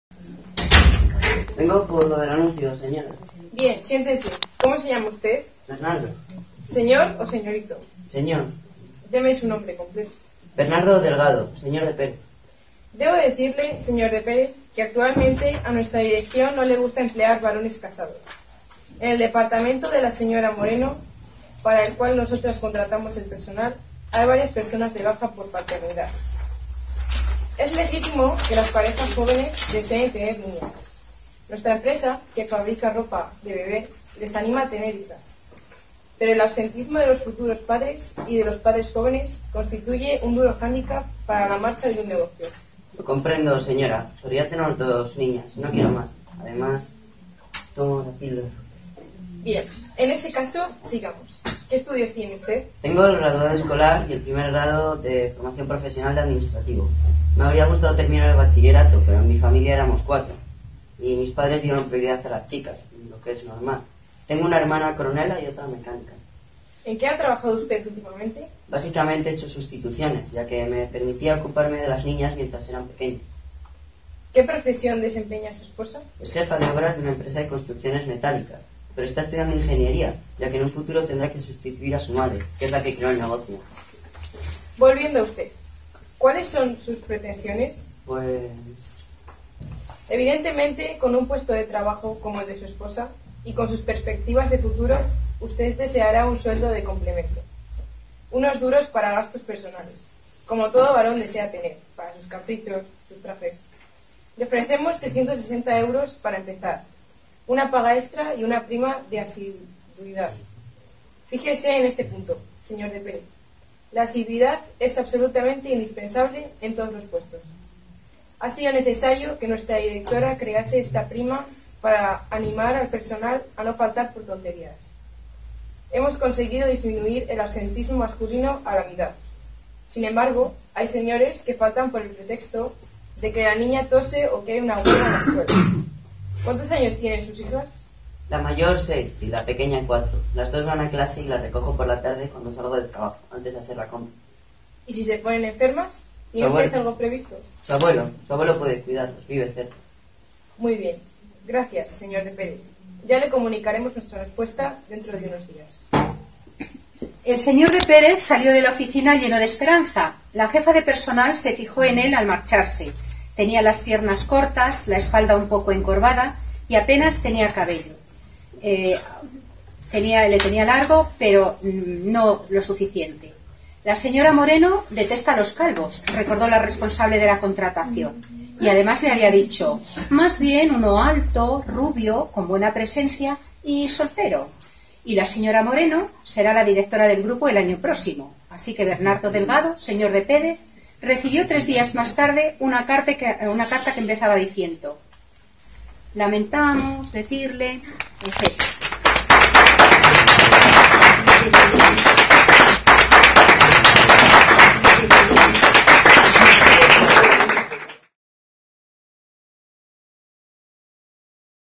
El mundo al revés
Dramatización realizada por alumnos de 4ºA de la ESO cuyo objetivo es reflexionar sobre las desigualdades de género y sacar conclusiones.

IES Galileo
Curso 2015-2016